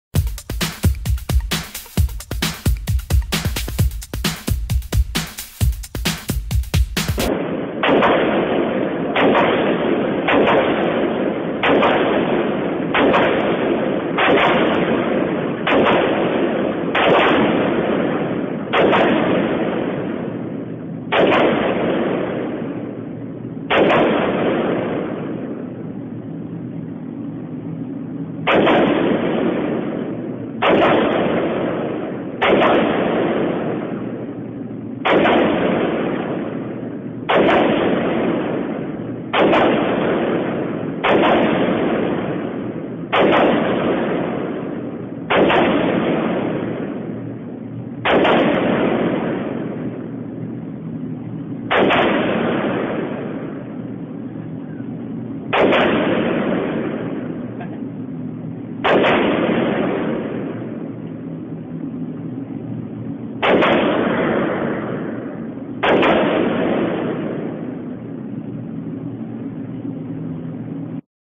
Ar 15 Practice Rami reloaded
Rami reloaded shooting a Ar 15